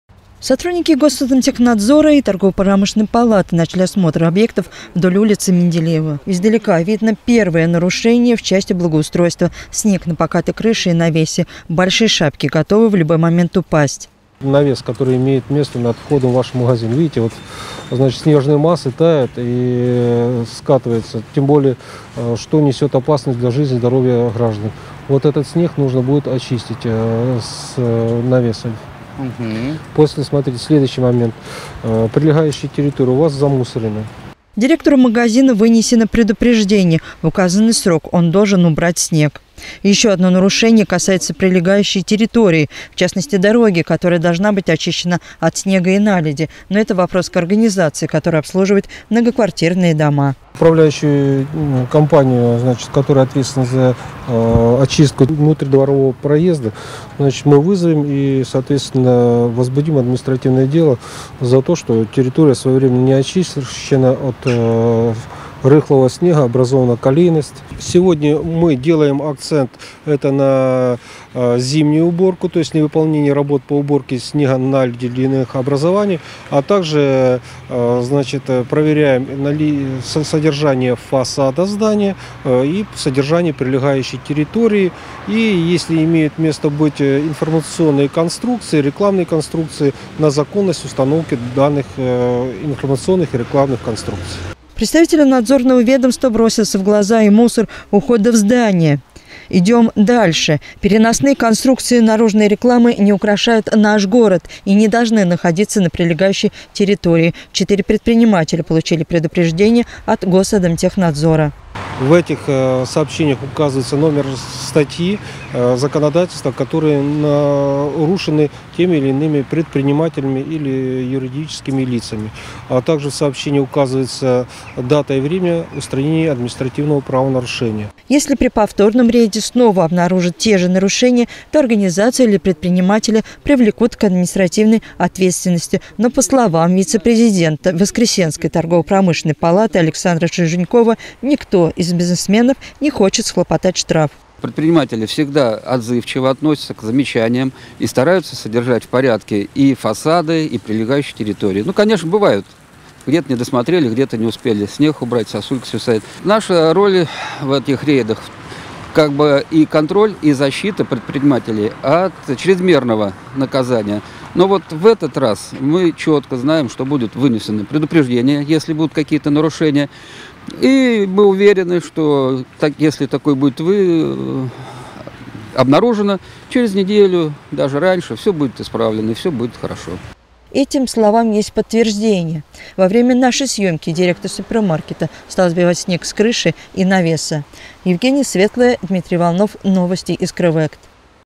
Снег на навесе, мусор у входа. Какие еще нарушения выявил Госадмтехнадзор?
Порядок возле магазинов должен быть всегда, но зимой к предпринимателям предъявляются дополнительные требования. Сегодня представители Госадмтехнадзора и Торгово-промышленной палаты проверили соблюдение норм на улице Менделеева. Вместе с ним в рейде участвовала и наша съемочная группа.